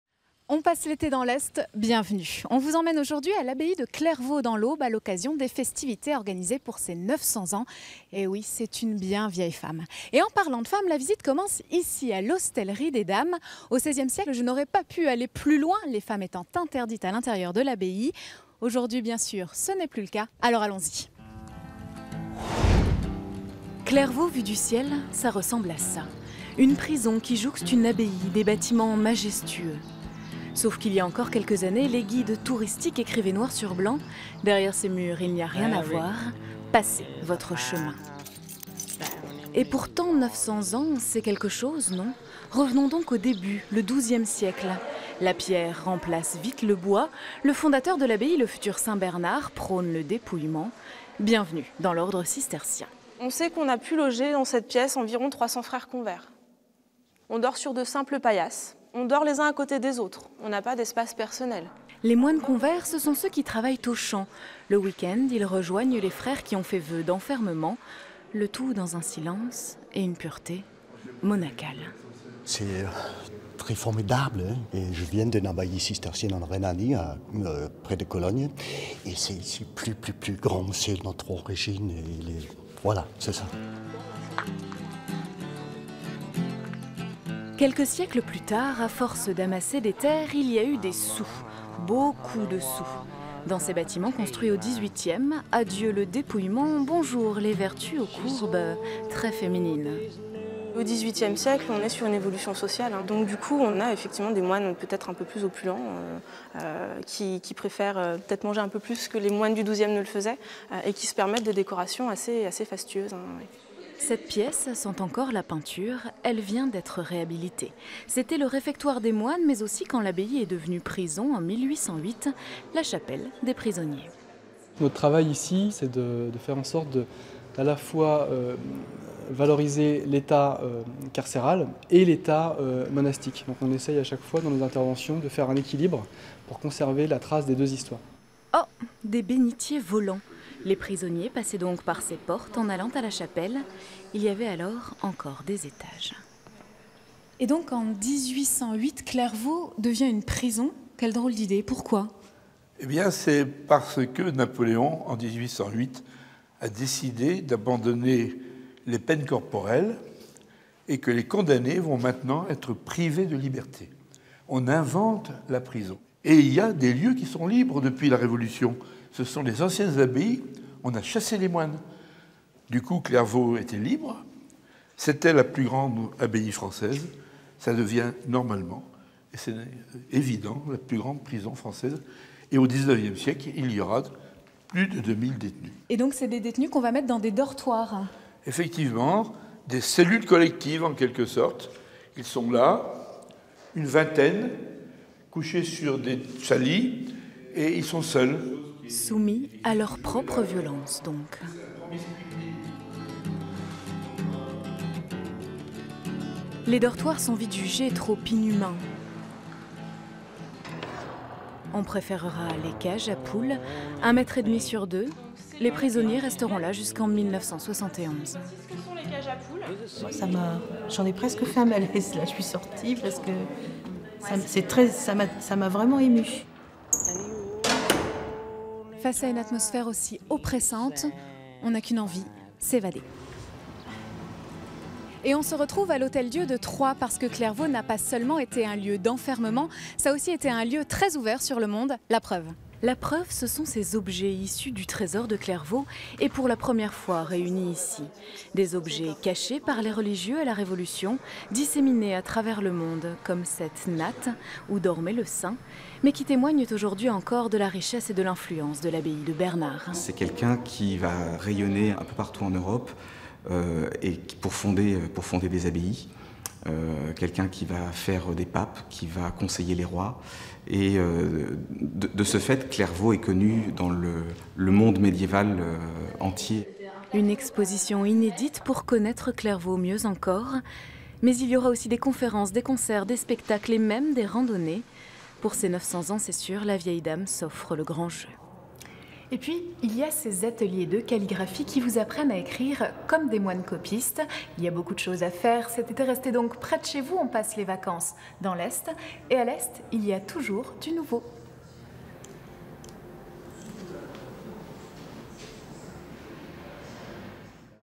900 ans de l'abbaye de Clairvaux
Votre émission cet été dans l'Est vous emmène cette fois-ci dans un endroit exceptionnel, un lieu aux mille visages, qui, en 900 ans d'existence, a connu pas moins de 5 vies : il s'agit de l'abbaye de Clairvaux dans l'Aube. Une abbaye toute en bois au départ, devenue un chef d'oeurvre de l'art gothique primitif, partie ensuite à la conquête du monde (ou tout du moins de la pensée occidentale), pour être transformée en prison, l'une des pires de France, racontait-on. Aujourd'hui, elle coule des jours heureux au sein de sa toute nouvelle vie culturelle… Et vous pouvez la visiter, cet été, de mille façons possibles : expos, concerts, spectacles, randonnées… Poussez avec France Champagne-Ardenne la porte de ce lieu incroyable !